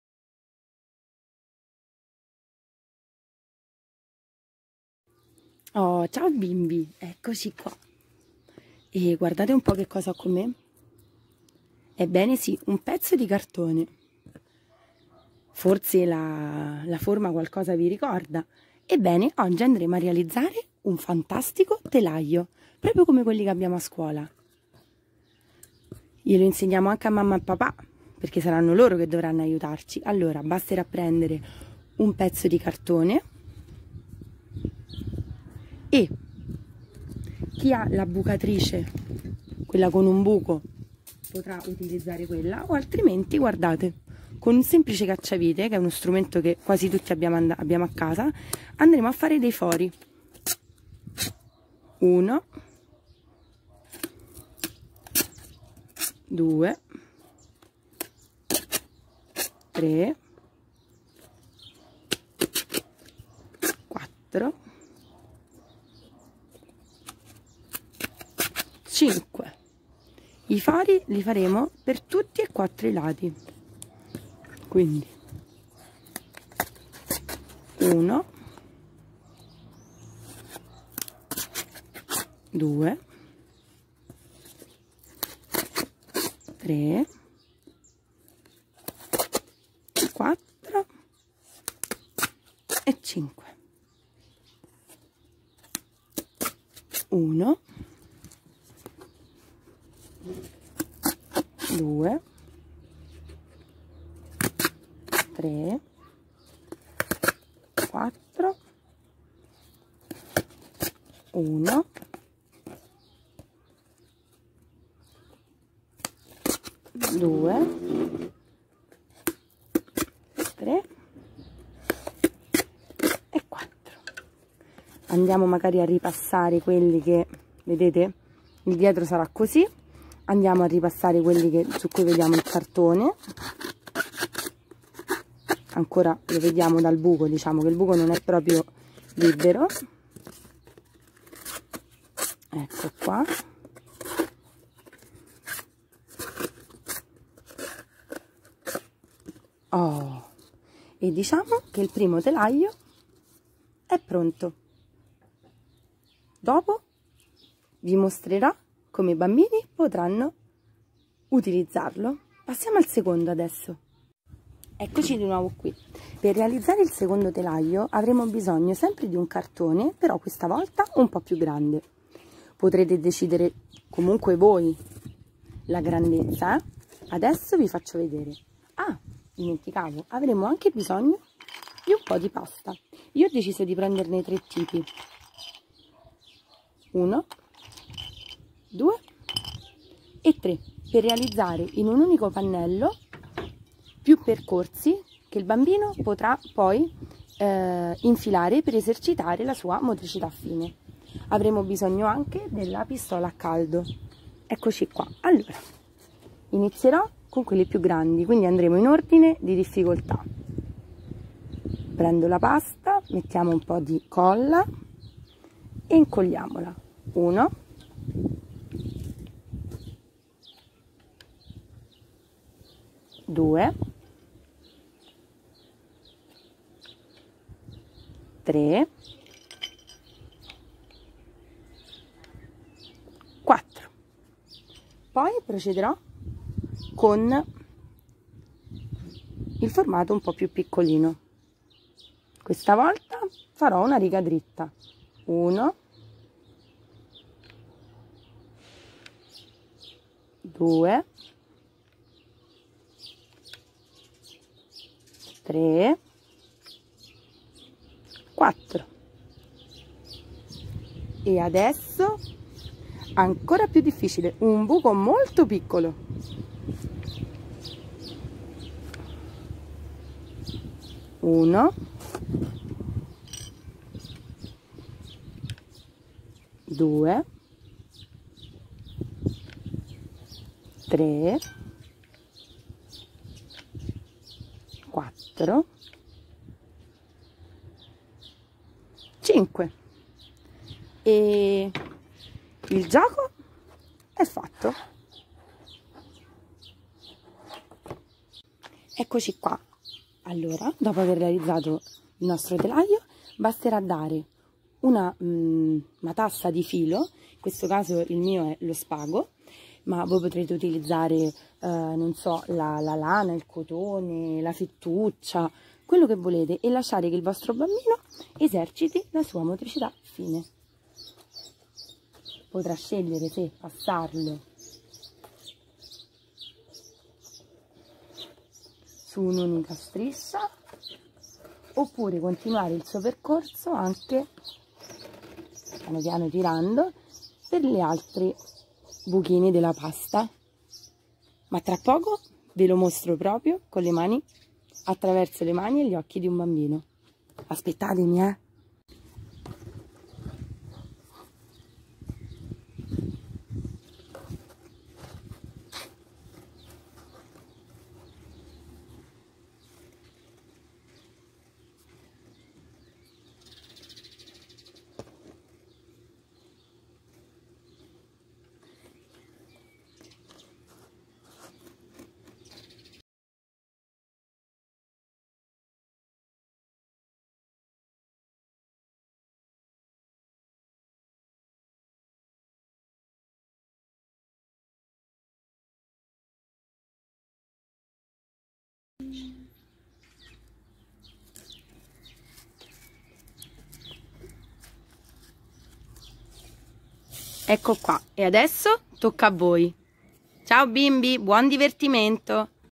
Alleniamo la motricità fine
Strumenti semplici ma efficaci.
Costruzione di due telai, per allenare la motricità fine dei bambini.